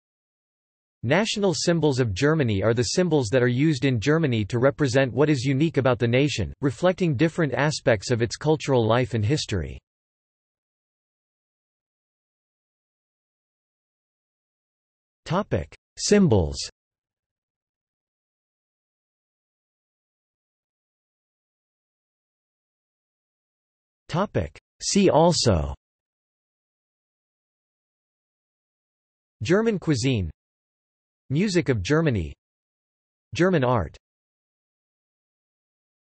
National symbols of Germany | Wikipedia audio article
This is an audio version of the Wikipedia Article:
National symbols of Germany

00:00:11 1 Symbols
00:00:20 2 See also

- Socrates

SUMMARY
National symbols of Germany are the symbols that are used in Germany to represent what is unique about the nation, reflecting different aspects of its cultural life and history.